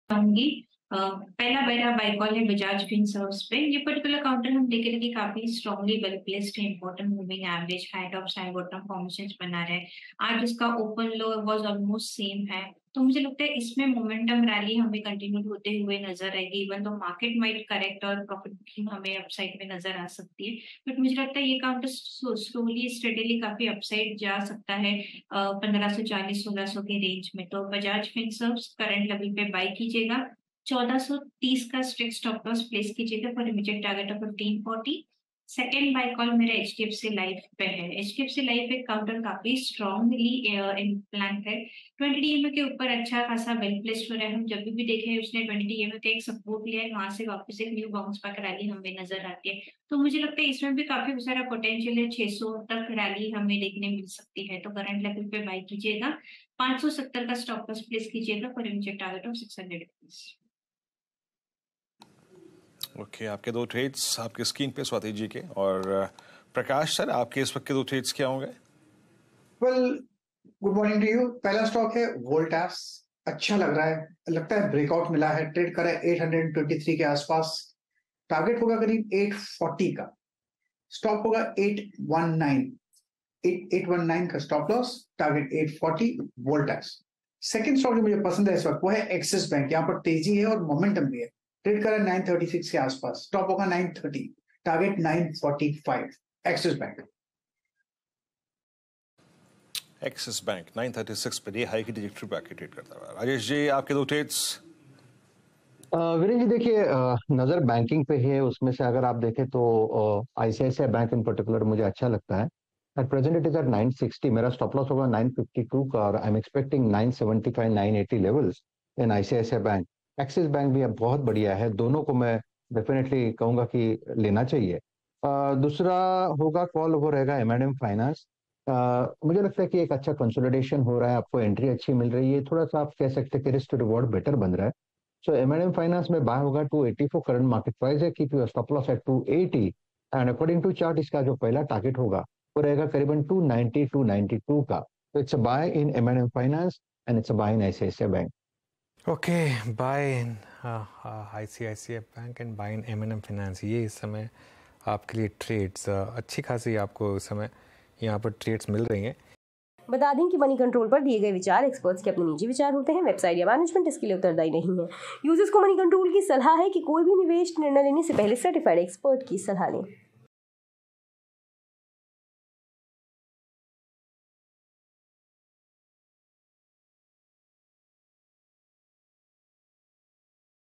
Bajaj Finserv Share Price: इस Stoploss के साथ करें खरीदारी || Hot stocks || stock to invest
MarketsWithMC | Bajaj Finserv Share में जानें निवेश को लेकर क्या है Experts की राय. जानें किन levels पर करनी चाहिए आपको खरीदारी और क्या है Target Price और जानिए किस Strategy के साथ इस Stock में पैसा कमाया जा सकता है. देखें वीडियो.